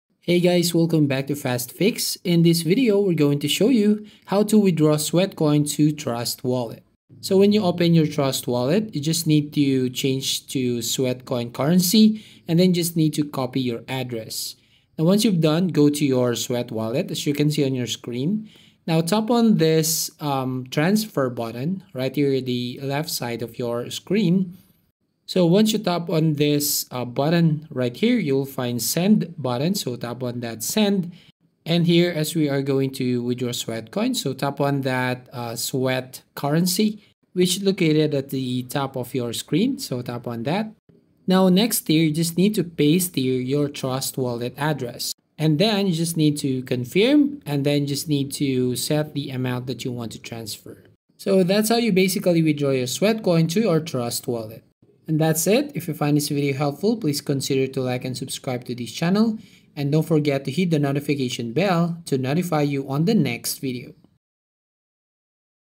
How To Withdraw Sweatcoin To Trust Wallet (Easy Guide)
In this video I'll show you How To Withdraw Sweatcoin To Trust Wallet (Easy Guide). The method is very simple and clearly described in the video. Follow all of the steps & you will know how to add Sweatcoin to trust wallet